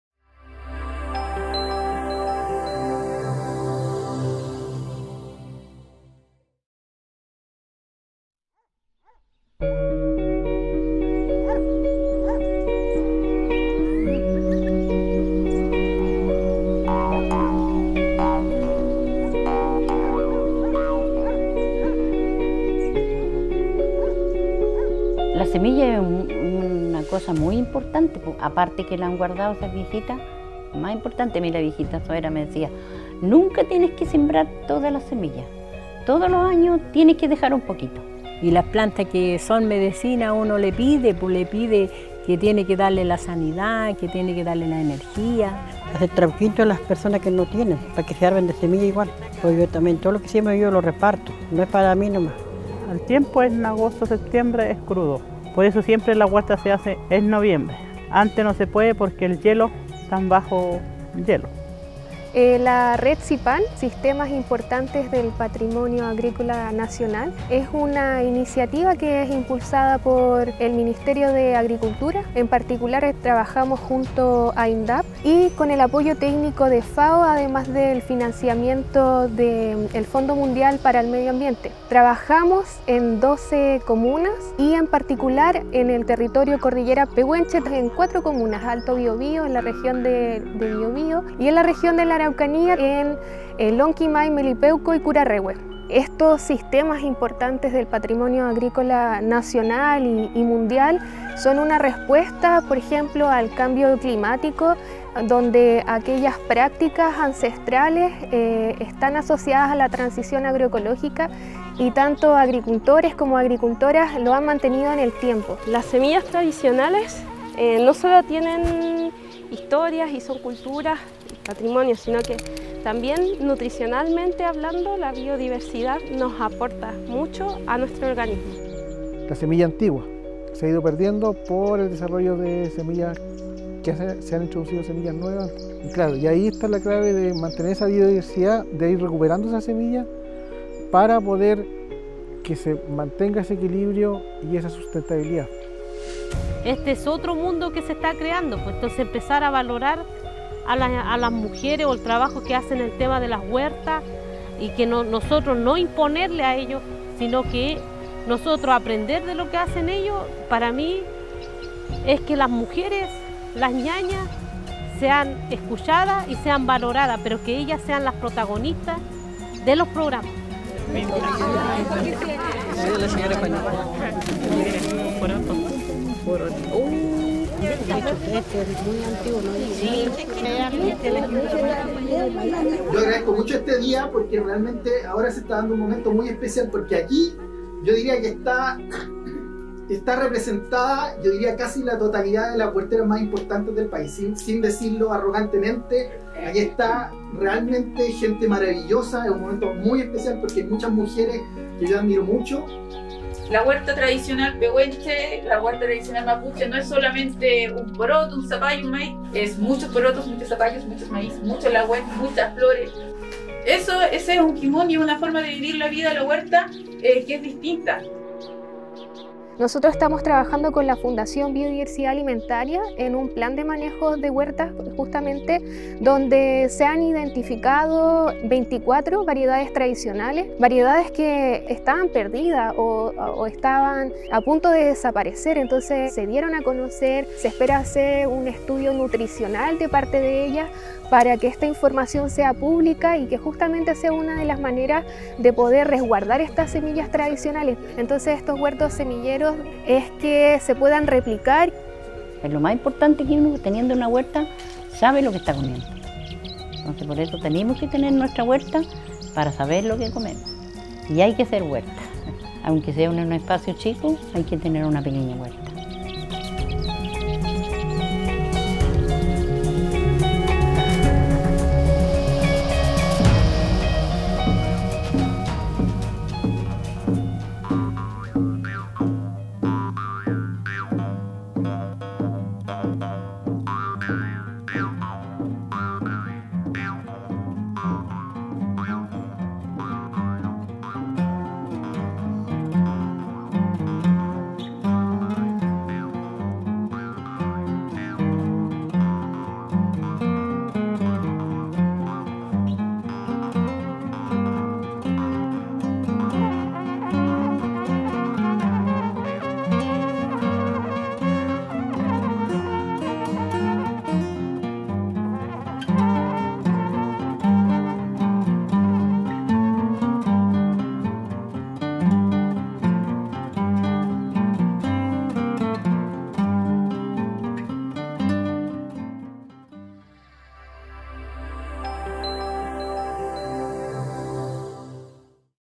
Huertas biodiversas de la Red SIPAN| Presentación
Este video introduce una serie de cuatro documentales sobre las huertas biodiversas presentes en la Cordillera Pehuenche, en el sur de Chile.

En las cuatro comunas donde se grabó esta serie, desde tiempos ancestrales las comunidades indígenas y campesinas han cultivado huertas de sorprendente biodiversidad, que conservan semillas y prácticas agrícolas tradicionales. En ellas, las cuidadadoras de semillas protegen este patrimonio genético y en esta serie conoceremos a cinco de estas mujeres: Juanita Faúndez, Berta Quiriban, Delfina Curriao y Aurora Marillan, junto a su hija Aurora Curical.

La serie documental es parte del proceso de visibilización y rescate de saberes que lleva a cabo la Red de Sistemas Importantes del Patrimonio Agrícola Nacional (Red SIPAN), que impulsa el Ministerio de Agricultura de Chile, a través de la Oficina de Estudios y Políticas Agrarias (ODEPA) y el Instituto de Desarrollo Agropecuario (INDAP).  Esta iniciativa cuenta con la supervisión y apoyo técnico de la FAO y financiamiento del Fondo para el Medio Ambiente Mundial.